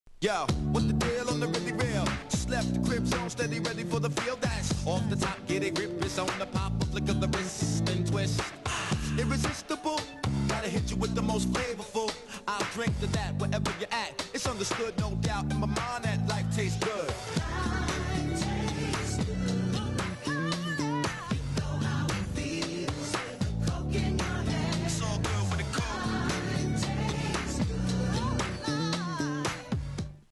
Coca Cola Ad - Life Tastes Good (DJ Mix)
Ripped from VHS.

Aired circa 2001 during Buggles to Bizkit, a MTV 20th anniversary program.